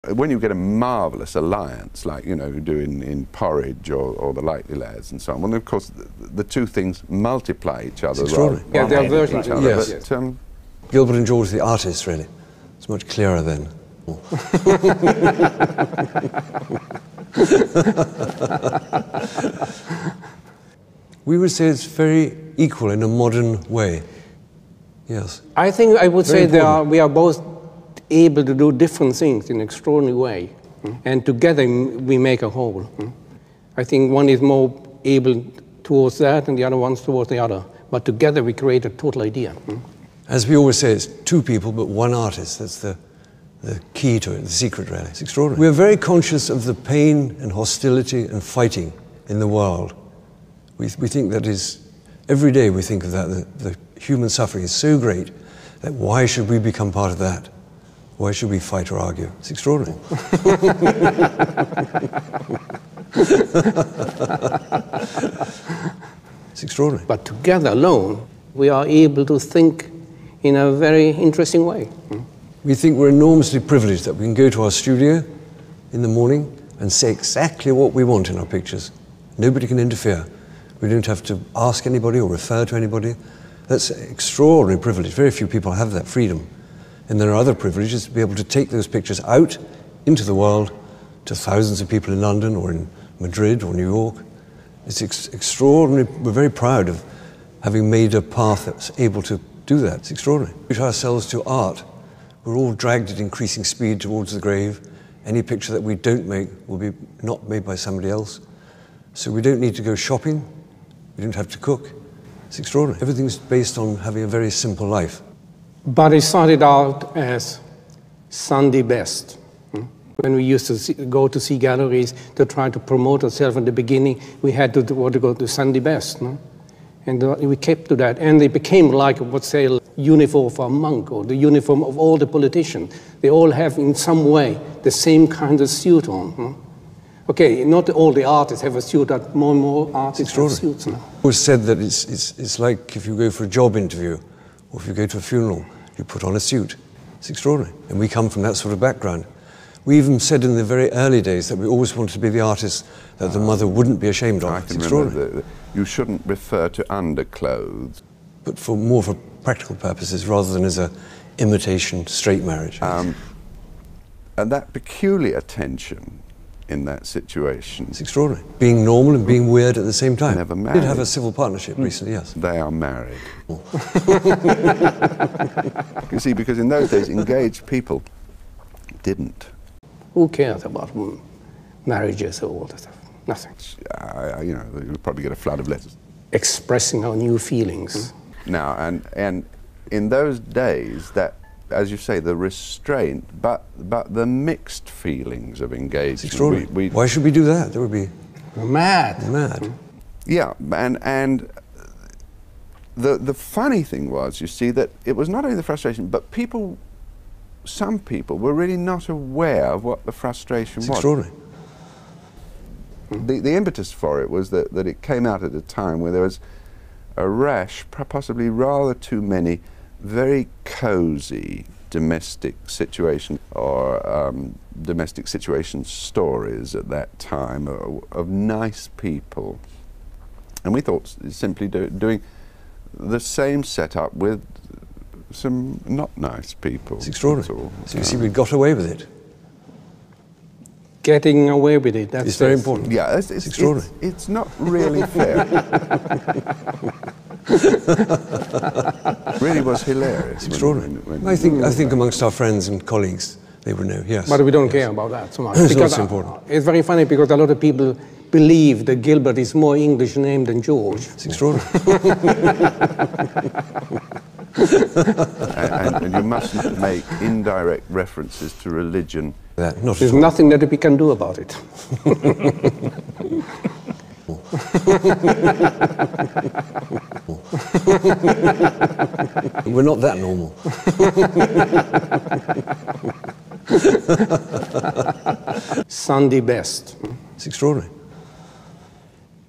Sunday best, Jay Jopling in an extraordinary interview with Gilbert and George.
White Cube art dealer, Jay Jopling, in his Sunday best talks to artists Gilbert and George by Derek Walker